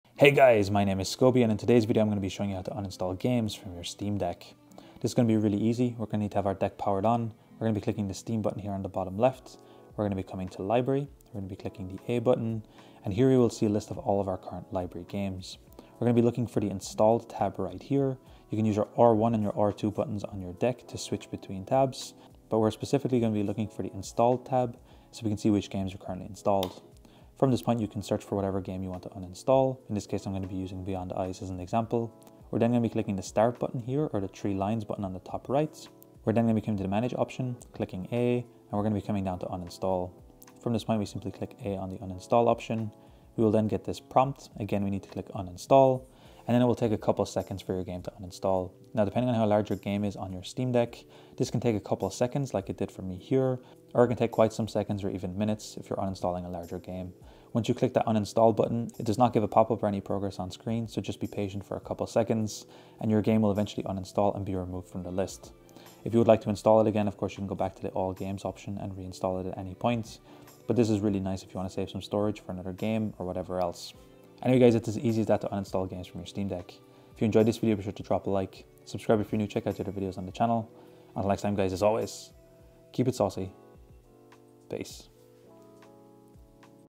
Uninstall Games on Steam Deck[Delete games from Steam Deck]!
Hey, guys, my name is Scoby Tech and in todays video I am going to be showing you how to uninstall games from your Steam deck!

0:00 Intro
0:10 Open your installed library
0:32 Expand option's menu to uninstall
1:14 Outro